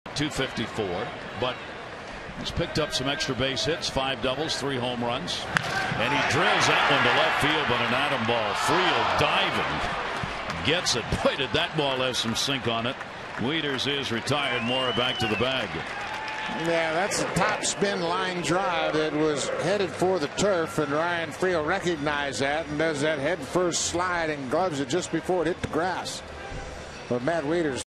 2009/07/27 Freel's diving catch
KC@BAL: Freel gets dirty on a great diving catch